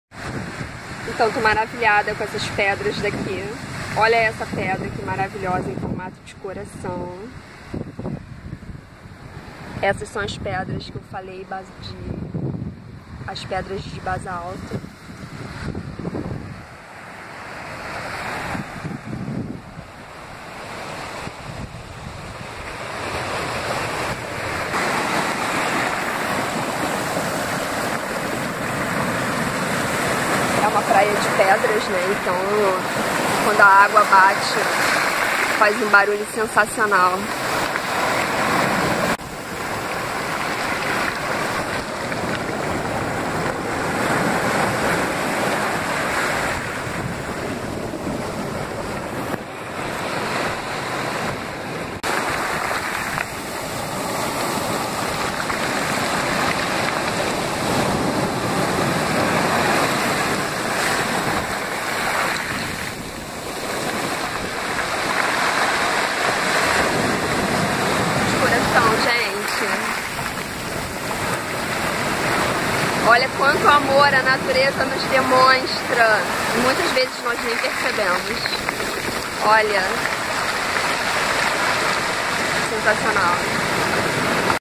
Praia Monte Verde - Portugal Pedras em formato de Coração
Olá pessoal,

Monte Verde é uma praia marítima localizada na vulcânica Ilha de São Miguel nos Açores, com um vasto areal que oferece momentos de descontração, serenidade e aventura aos amantes dos esportes de mar. Muito conhecida pelas ondas que proporciona, ideal para o surf e bodyboard.

Para além de ser uma das praias com maior potencial, muito procurada por banhistas e palco de um dos maiores festivais de música – o Monte Verde Festival.

Atenciosamente,
Luciana Sousa